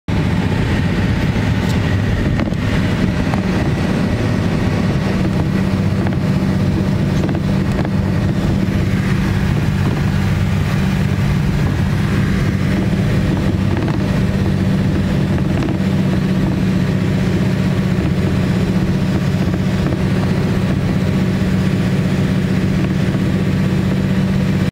Ялта - Симферополь - трасса, туман.
Вот такой туманище был по дороге и в Сказке (в верховьях Ялты).